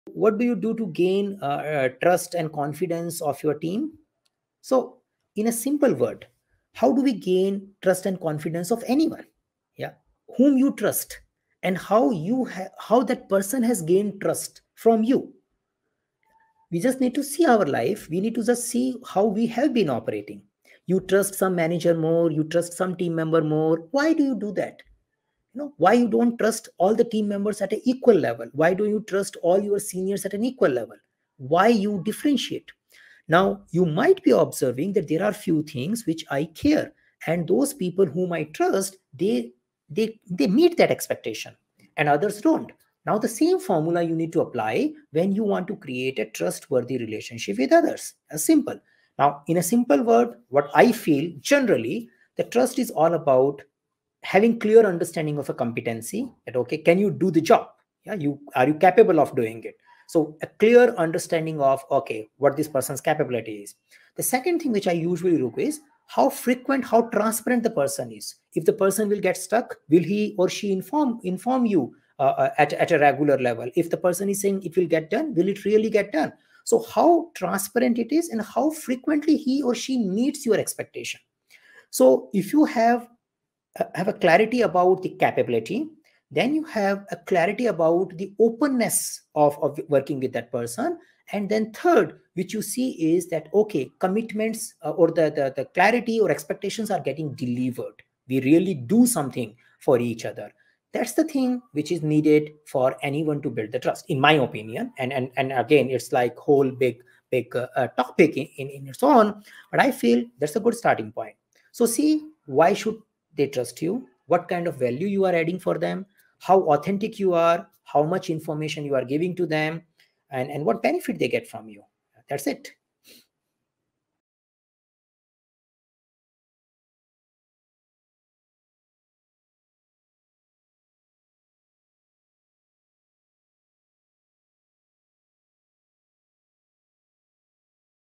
How Do You Gain Trust from the Team?
We all know that trust is dependent entirely on the behavior of others. If you have trust in someone, you will rely more easily on them. A team that lacks trust isn't really a team at all: it's just a collection of individuals collaborating, frequently with poor results. Trust is essential as it provides a sense of safety and boosts team productivity.

Here is our trainer, Mr Saket Bansal, sharing his idea through this video about how you can gain Scrum team confidence. According to him, we should be particular about certain things such as:

Why should the team trust you?
What kind of value are you adding to your team?
How authentic you really are?
How much information are you giving to them?
What kind of benefits do they get from you?

If you will have clear communication with a trustworthy relationship, then your team will definitely enjoy a culture of honesty, psychological safety, and mutual respect.